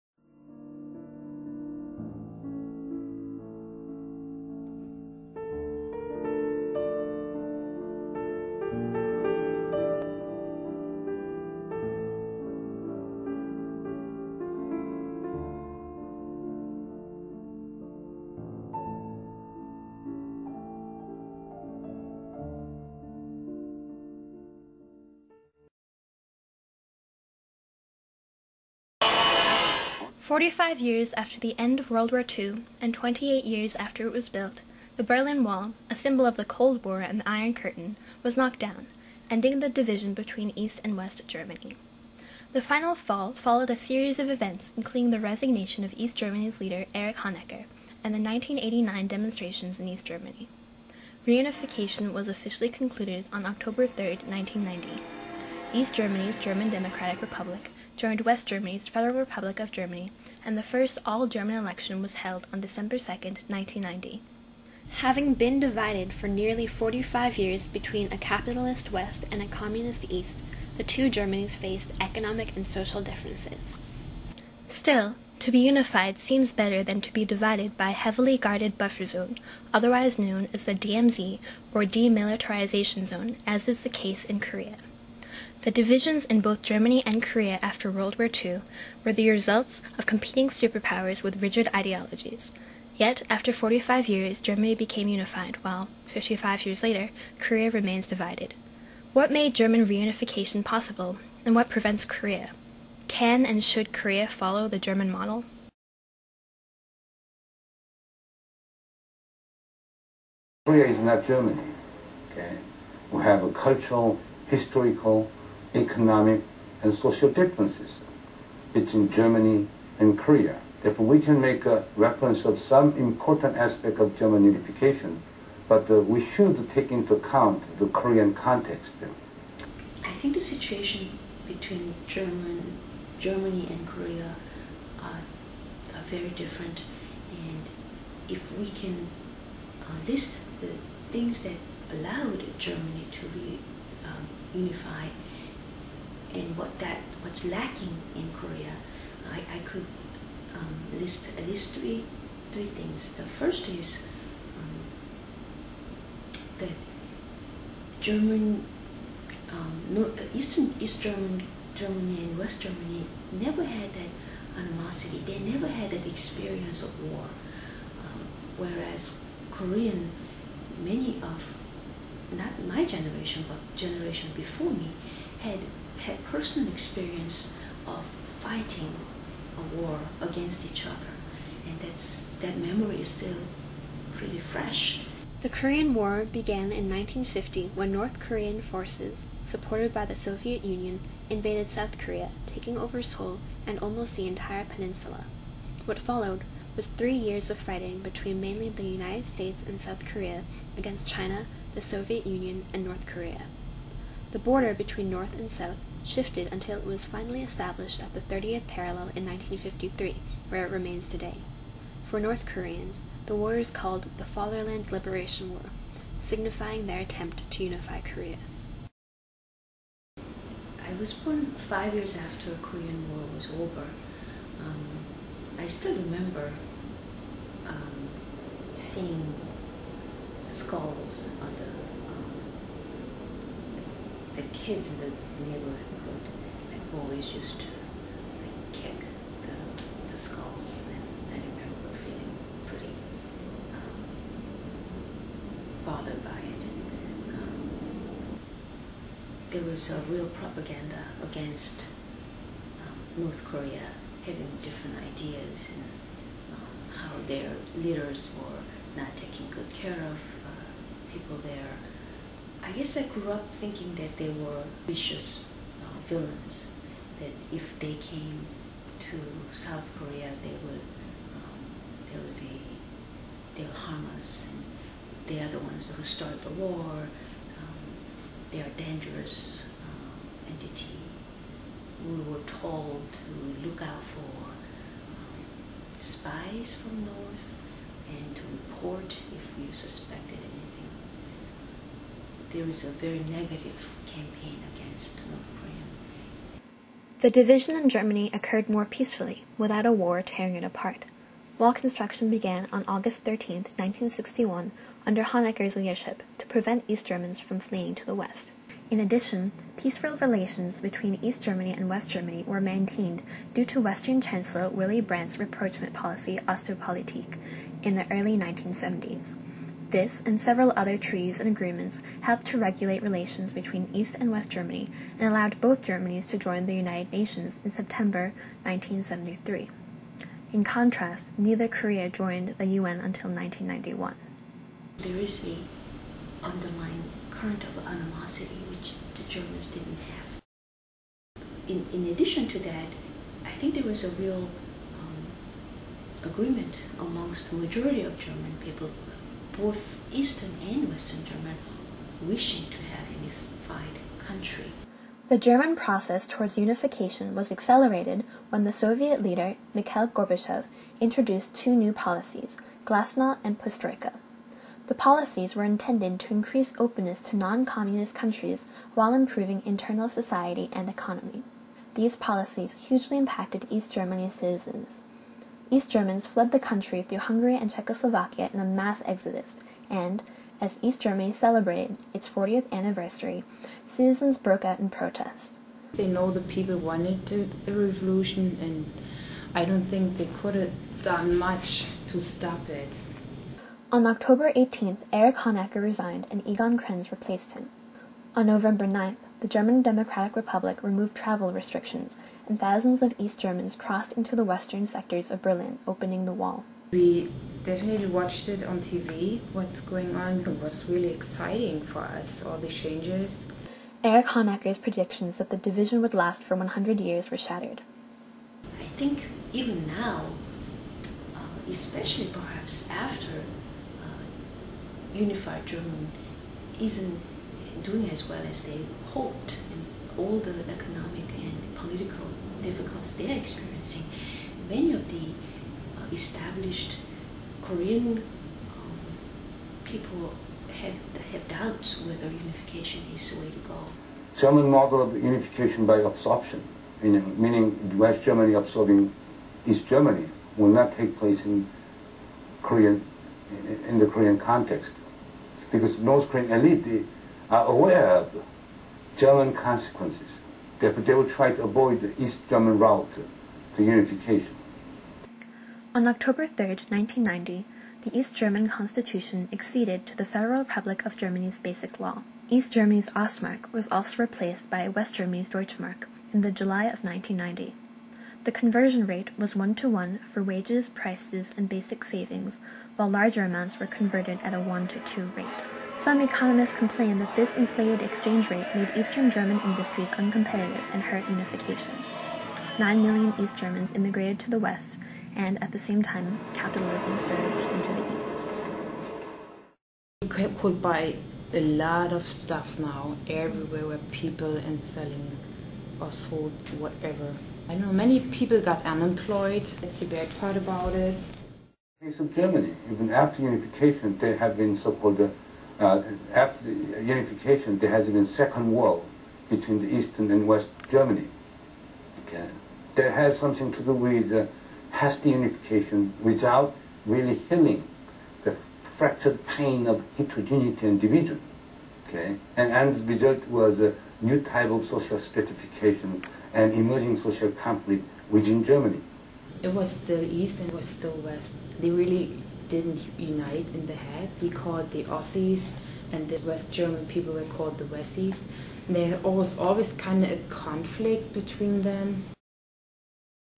Germany Unified, Korea Divided
My final project for AP European History. A documentary comparing two nations.